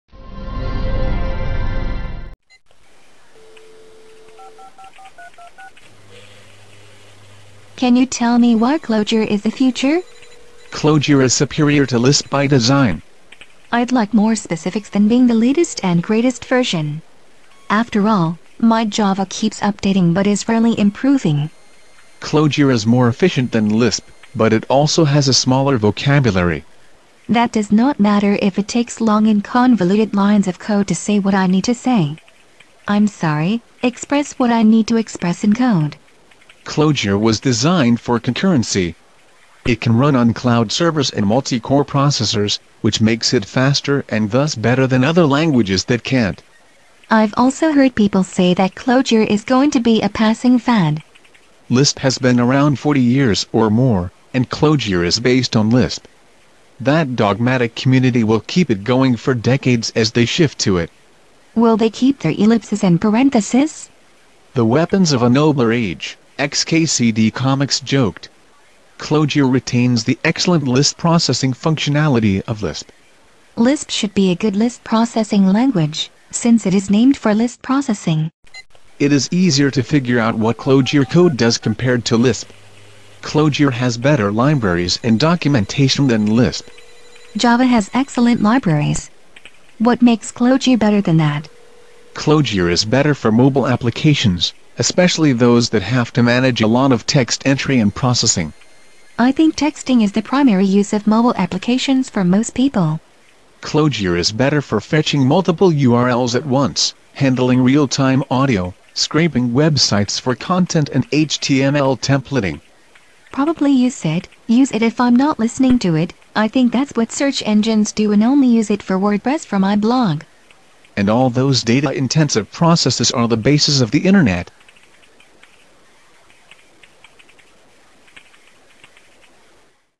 Why Clojure is the Future
Can you tell me why Clojure is the future?

Clojure is superior to Lisp by design.

I'd like more specifics than being the latest and greatest version. After all, my Java keeps updating but is rarely improving.

Clojure is more efficient than Lisp, but it also has a smaller vocabulary.

That does not matter if it takes long and convoluted lines of code to say what I need to say. I'm sorry, express what I need to express in code.

Clojure was designed for concurrency. It can run on cloud servers and multi-core processors, which makes it faster and thus better than other languages that can't.

I've also heard people say that Clojure is going to be a passing fad.

Lisp has been around forty years or more, and Clojure is based on Lisp. That dogmatic community will keep it going for decades as they shift to it.

Will they keep their ellipses and parenthesis?

The weapons of a nobler age, XKCD comics joked. Clojure retains the excellent list processing functionality of Lisp.

Lisp should be a good list processing language, since it is named for list processing.

It is easier to figure out what Clojure code does compared to Lisp. Clojure has better libraries and documentation than Lisp.

Java has excellent libraries. What makes Clojure better than that?

Clojure is better for mobile applications, especially those that have to manage a lot of text entry and processing.

I think texting is the primary use of mobile applications for most people.

Clojure is better for fetching multiple URLs at once, handling real time audio, scraping websites for content and HTML templating.

Probably use it, use it if I'm not listening to it, I think that's what search engines do and only use it for Wordpress for my blog.

And all those data intensive processes are the basis of the internet.